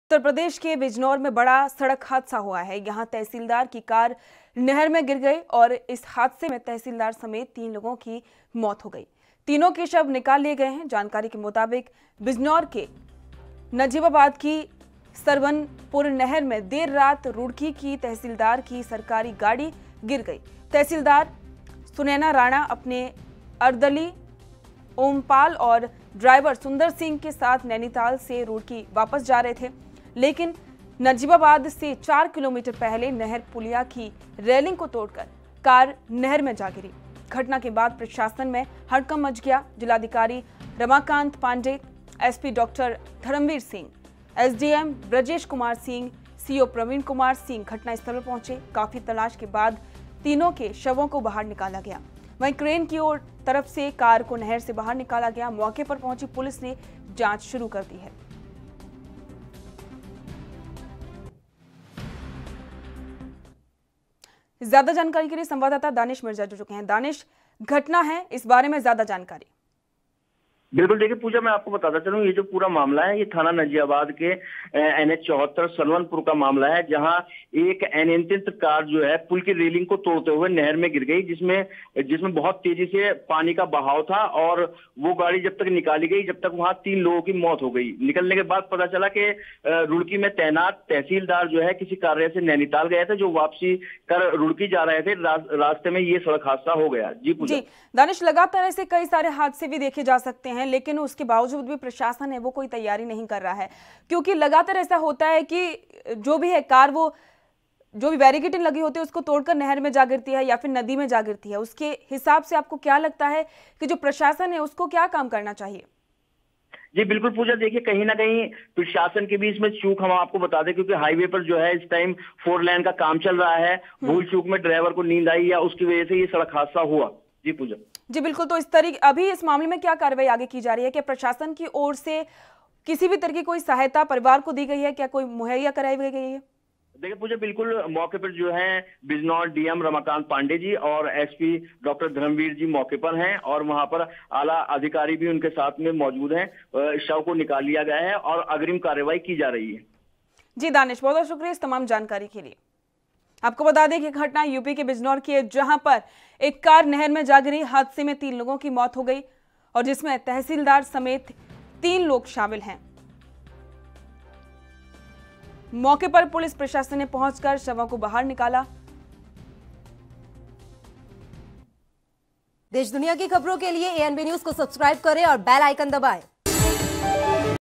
Bijnor में हुआ बड़ा सड़क हादसा, तहसीलदार समेत 3 की मौत | ANB NEWS
Bijnor में हुआ बड़ा सड़क हादसा, तहसीलदार समेत 3 की मौत |